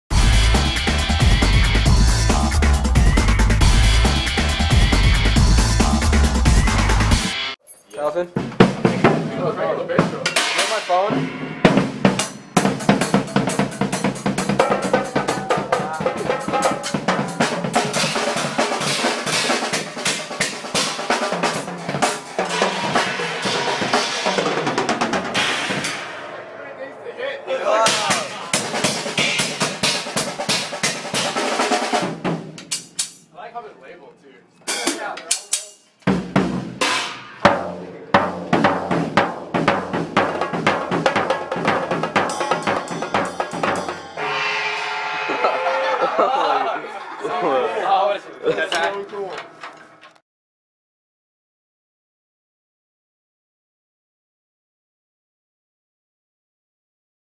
Terry Bozzio's drum set | Played by Kaz Mogi
I was fortunate enough to go the DW factory with my students from Lakewood High School. We got a tour of the facility and as a special bonus, we were able to see and play Terry Bozzio's drum set! I've seen pictures and videos of his set, but it's a much different experience seeing it in person. It's so intimidating! I was playing jibberish. I was not worthy!